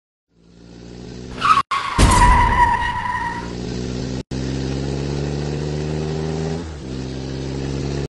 Car Crash - Stop Motion Animation
A stop motion animation, made during our after school animation club by children aged between 7-11.